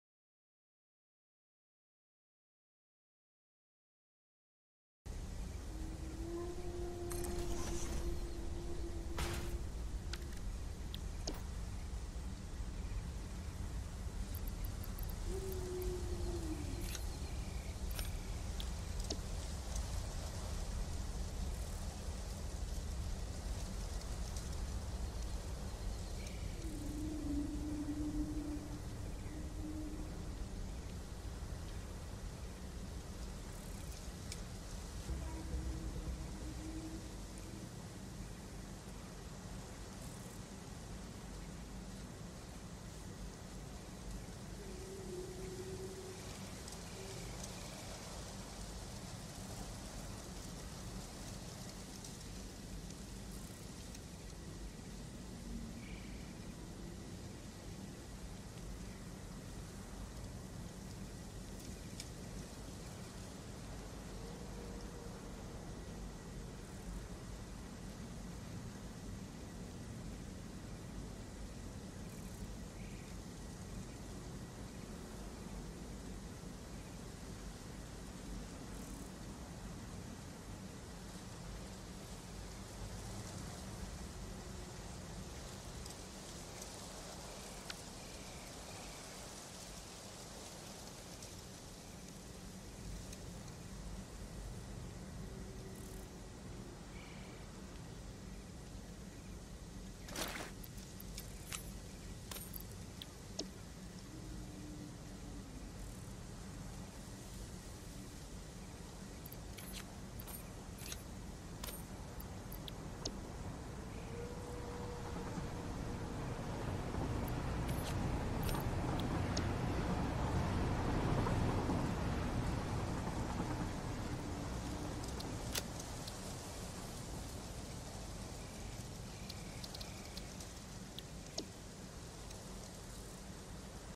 Wiremod storage box
storage container that shows what item is in there and how many